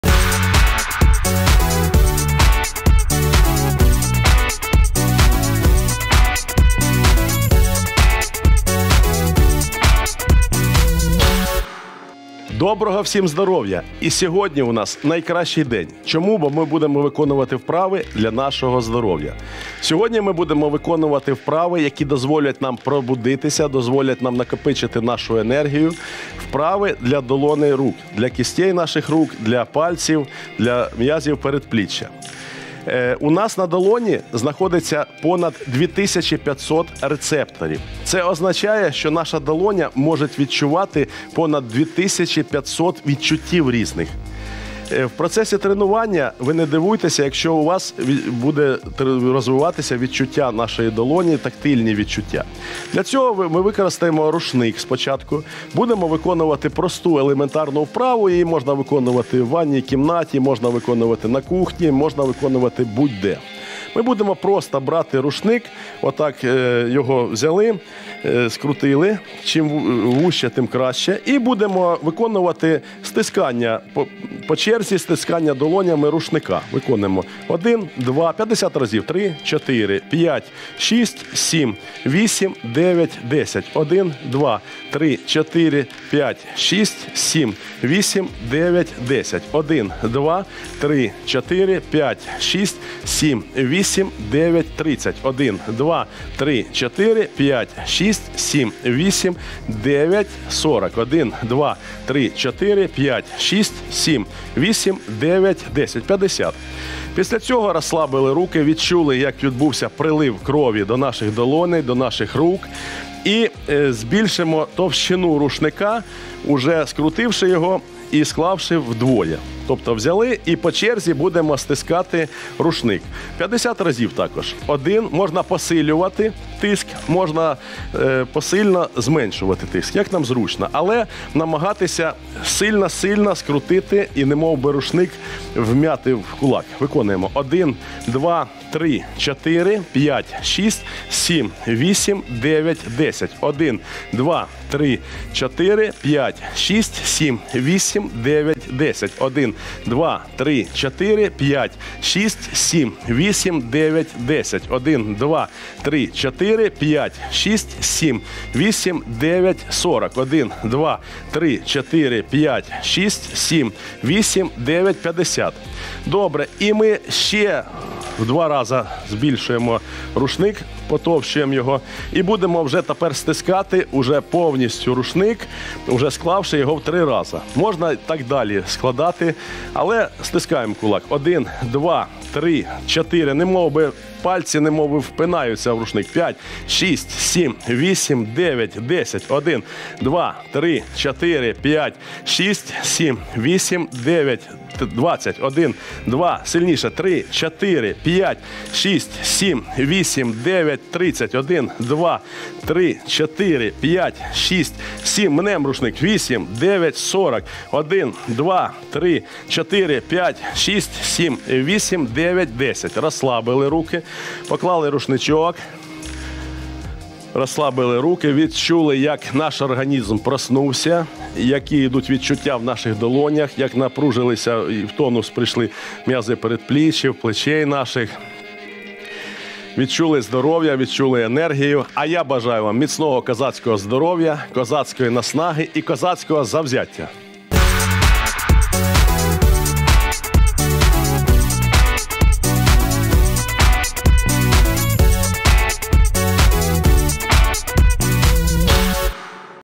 Зарядка Вправи для долоней предпліччя Притула
Сегодня будем выполнять упражнения, которые позволят нам пробудится, накопить нашу энергию. Сегодня - упражнения для ладоней рук, кистей, пальцев, мышц предплечья с помощью полотенца.